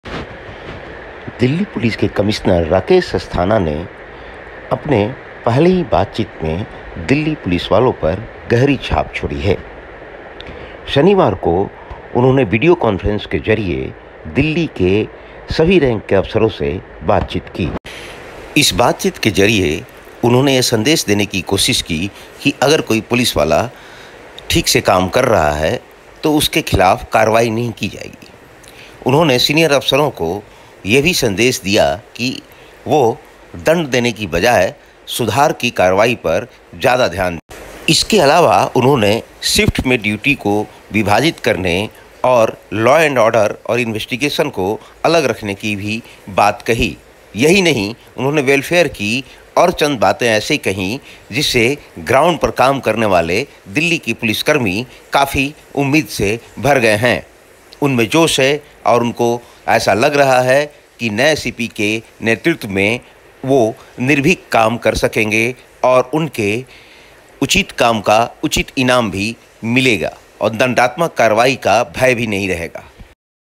दिल्ली सीपी राकेश अस्थाना की इन बातों से जोश में है पुलिसवाले policemen excited by these words of CP
इस वीडियो में हमने बताया है कि शनिवार को पहली बार दिल्ली के सभी पुलिसकर्मियों से मुखातिब हुए दिल्ली पुलिस कमिश्नर राकेश अस्थाना के किन बातों से दिल्ली के पुलिसकर्मी जोश में हैं। दरअसल राकेश अस्थाना ने ओपेन हाउस, दंड की बजाय सुधार जैसे कई संकेत दिए हैं जिससे पुलिसकर्मी उत्साह में हैं
In this video, we have told that for the first time on Saturday, the Delhi Police Commissioner Rakesh Asthana, who met all the policemen of Delhi, are excited about the things of Delhi Police. Actually Rakesh Asthana has given many signs like open house, reform instead of punishment, due to which the policemen are excited.